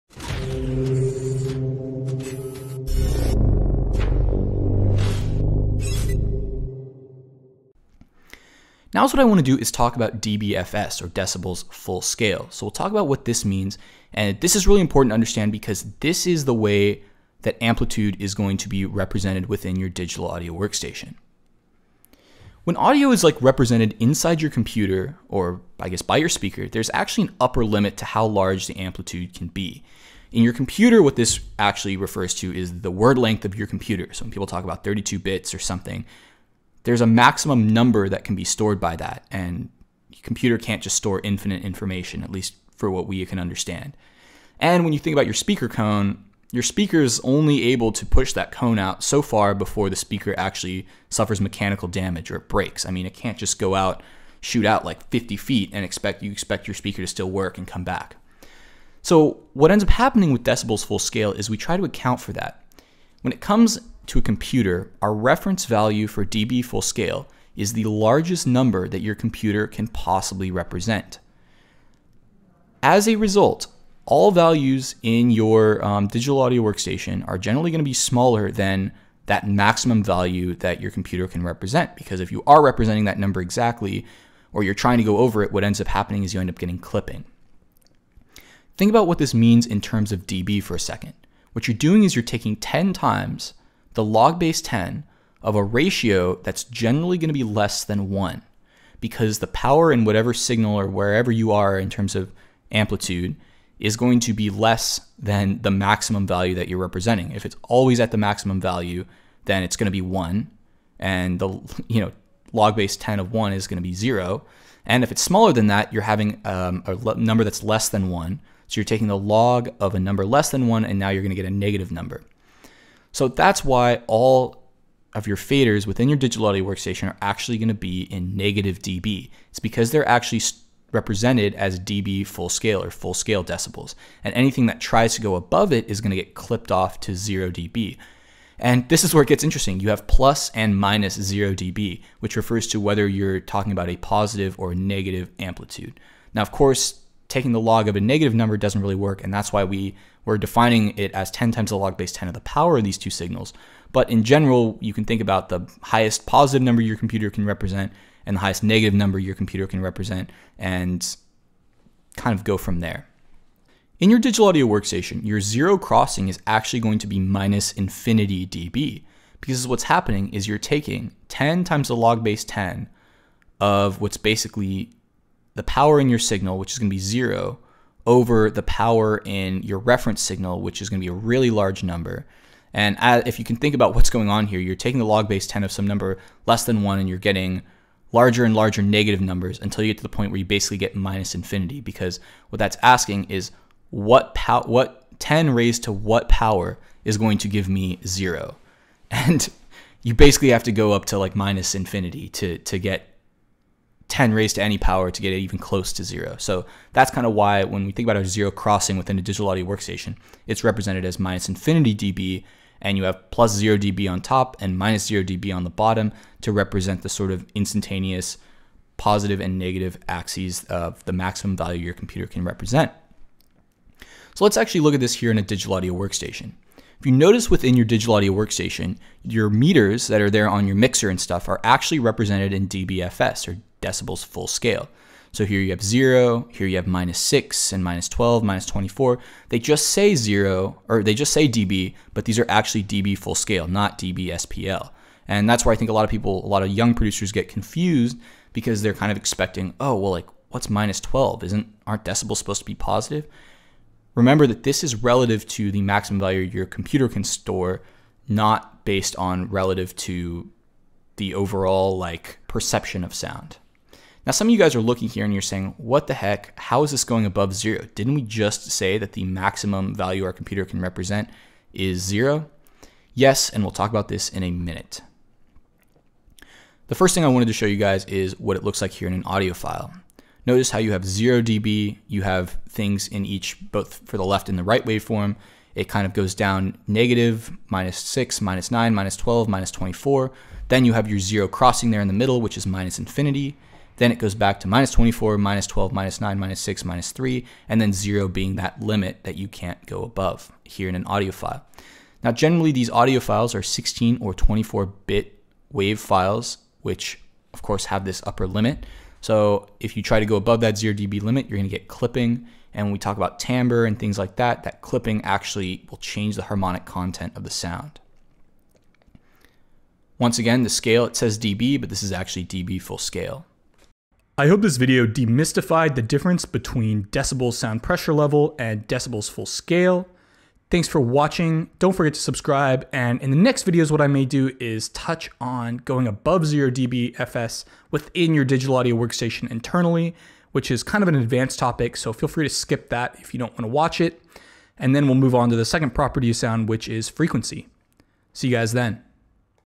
Intro to Music Production [Module 3.6] - Measuring Amplitude in dBFS
In this video, we will talk about measuring amplitude in dBFS, or Decibels Full Scale, which is the way amplitude is measured within your computer or DAW